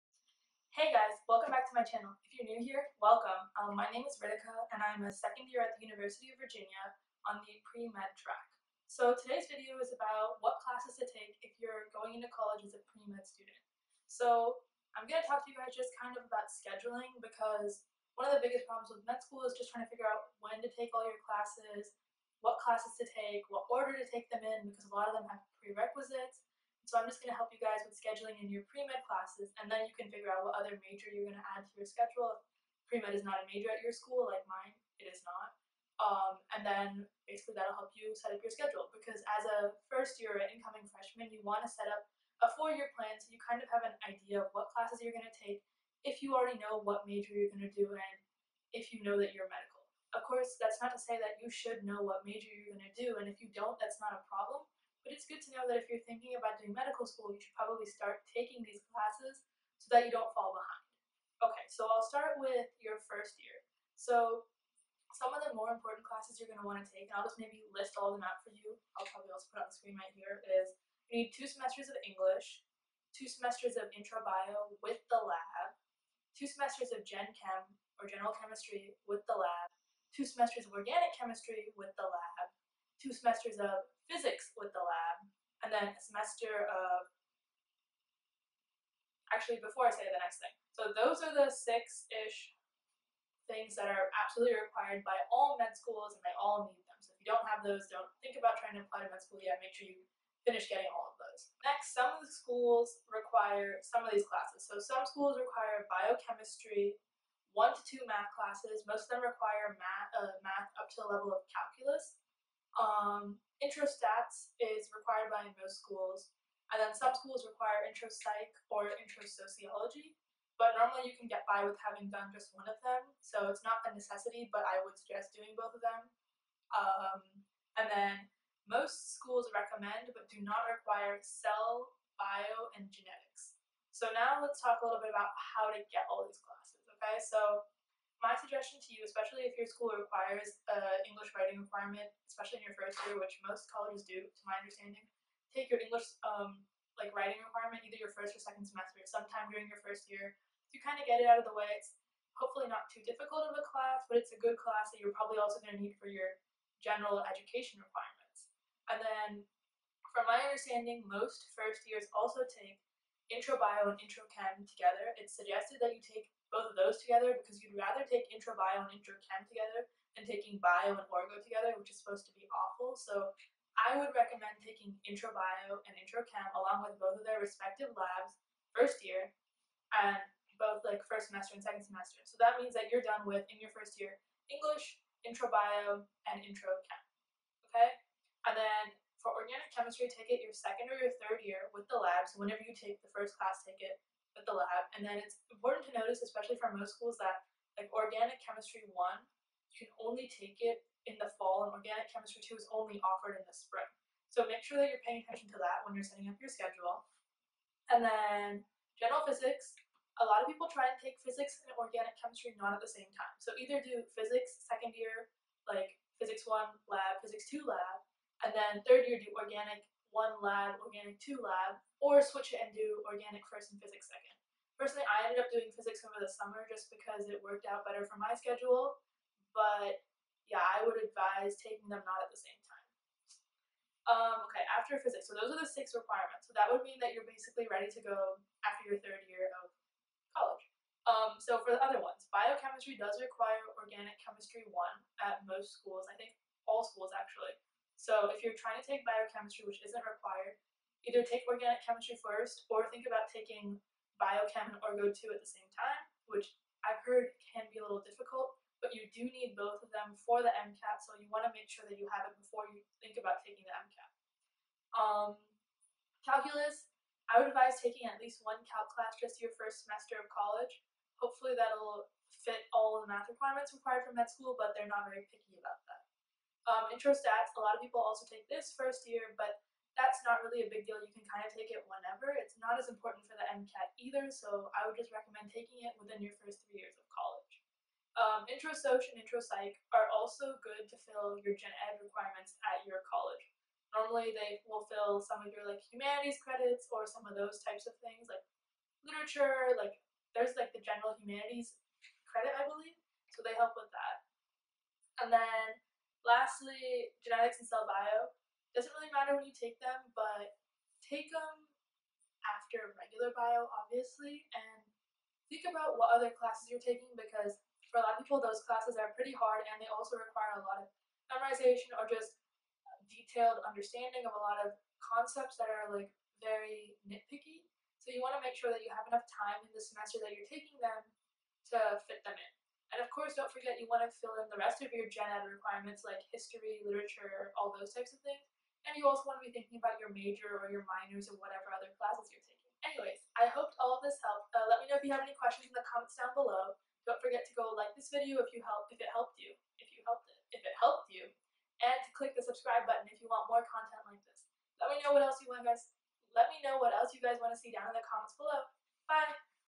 Premed Schedule | Planning What Classes to Take & When | UVA
how to create a 4 year plan to succeed as premedical undergraduate. what classes do you need, what is suggested and when should you take them?

In this video, I share with you guys what classes you need to take to succeed as a premed student. Regardless of your major, you will need to incorporate these classes and then some more that are suggested by medical schools. I also share how to fit all of them into a schedule that is not overwhelming!